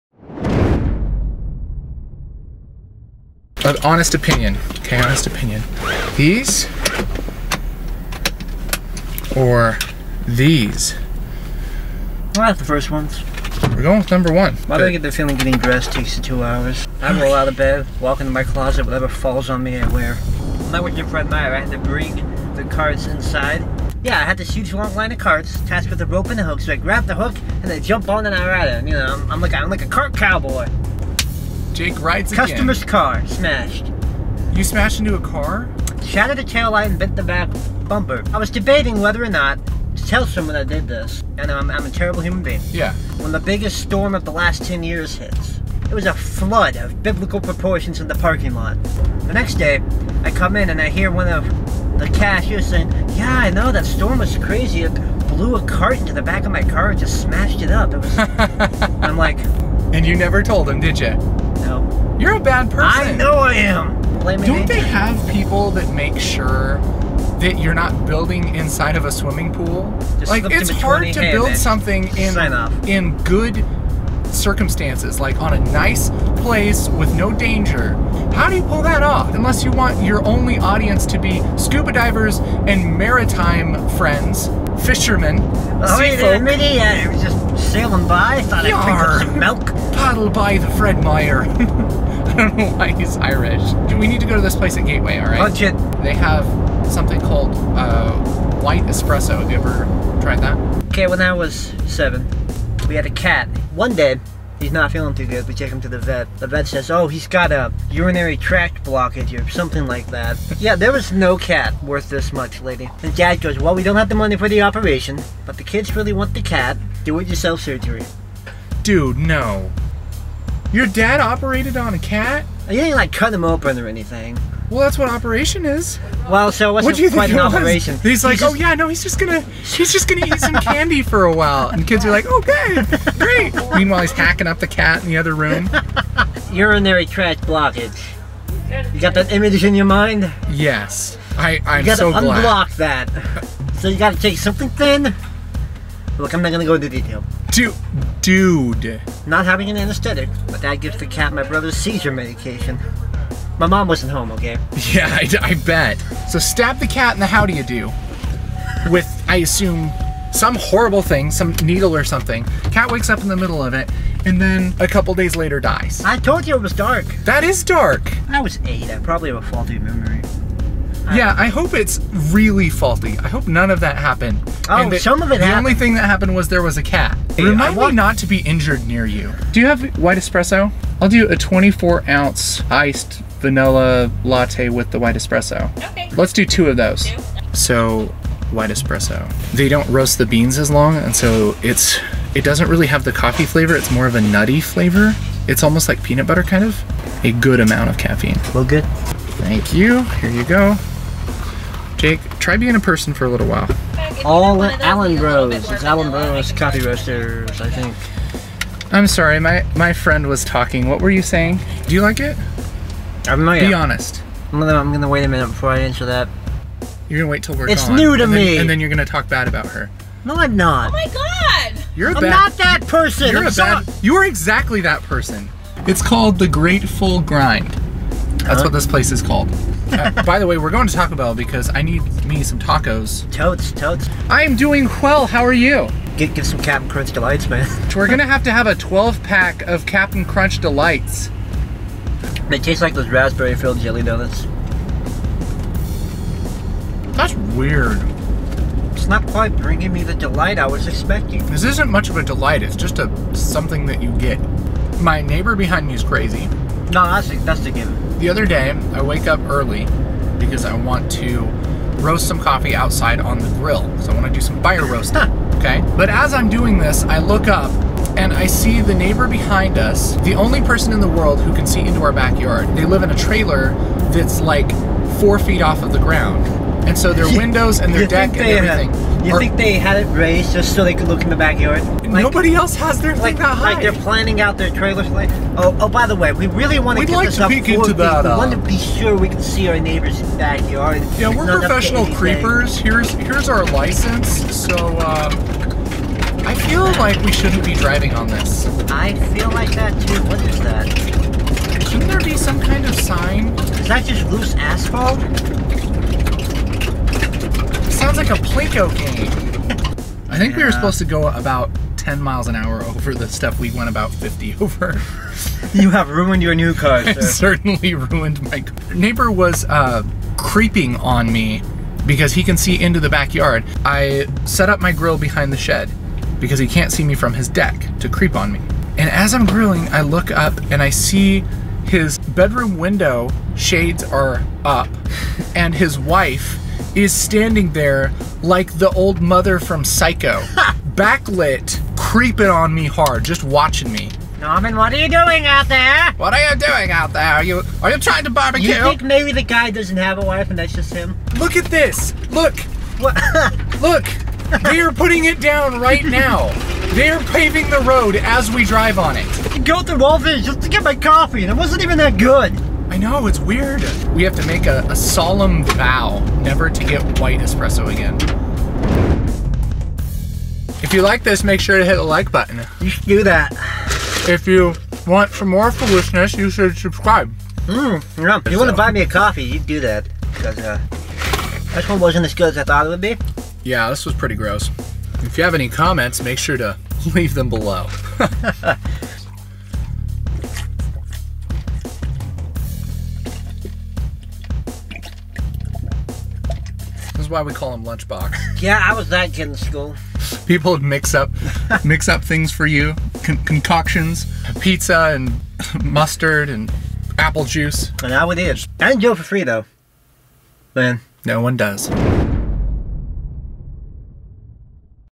E01: "Cats, Neighbors, and White Espresso" - Coffee Run
Today we talk criminal mischief, DIY surgery, and creepy neighbors.

Coffee Run.  Jake and Casey go get coffee, and discuss life's most troubling questions.